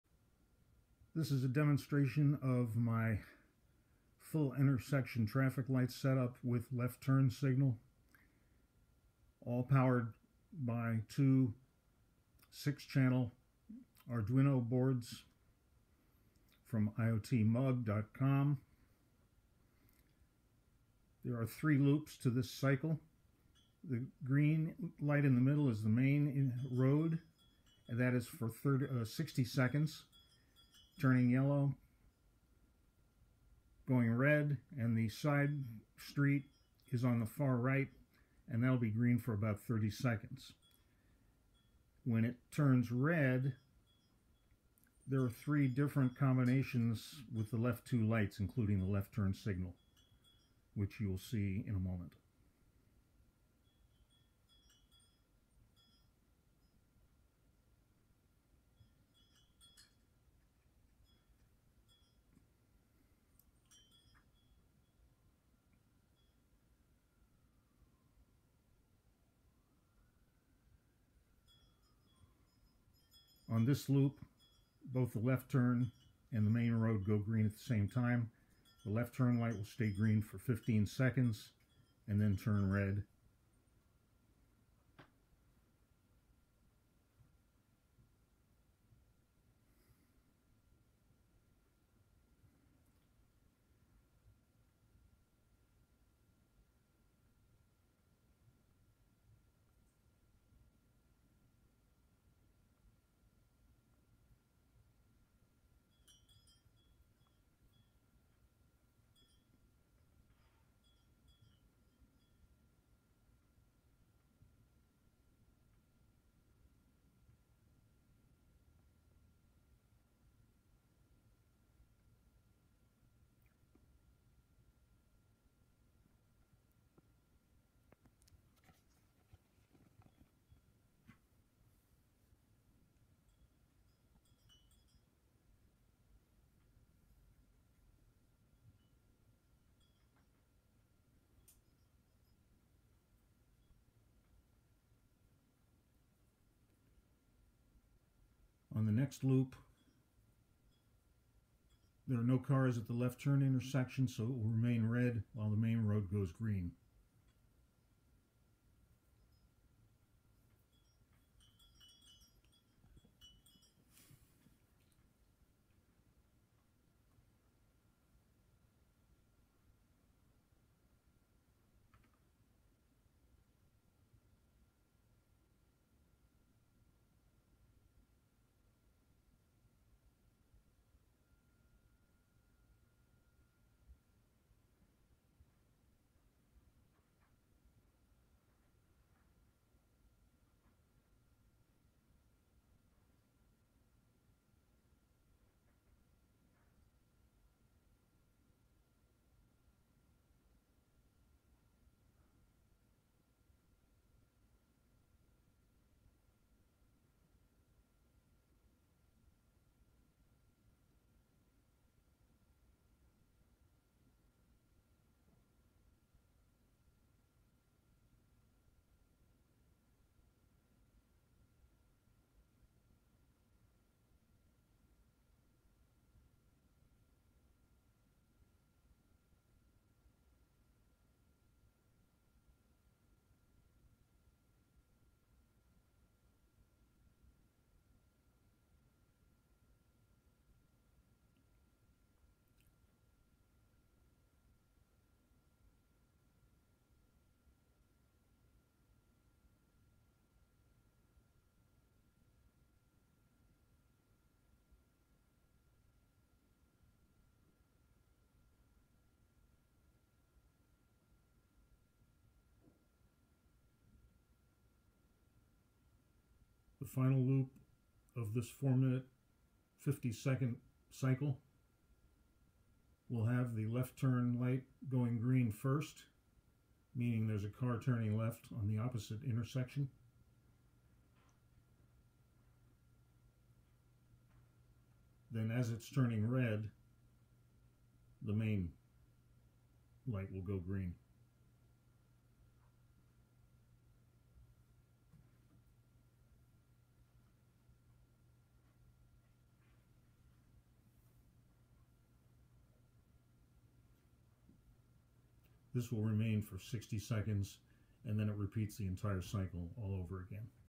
9 Channel Traffic Light Sequencing / Intersection Thanks for Customer Richard.
YouTube Video Description (Improved):

In this video, I'm demonstrating a full intersection traffic light system with a left turn signal, powered by 10 Channel Arduino boards from IoTMug.com. The system operates on a three-loop cycle, controlling traffic flow for the main road and a side street.

The green light for the main road is active for 30 to 60 seconds, before it turns yellow and then red. The side street's green light lasts for about 30 seconds. During this cycle, the left-turn signal is incorporated into three different scenarios:

First Loop: Both the main road and left turn light go green simultaneously. The left turn signal stays green for 15 seconds before turning red.
Second Loop: If no cars are waiting at the left turn, the left turn light stays red while the main road remains green.
Final Loop: As cars approach the left-turn intersection, the left-turn light goes green first. Once it turns red, the main road light turns green for 60 seconds.
This system repeats its cycle continuously, ensuring efficient traffic management.

Here is our product link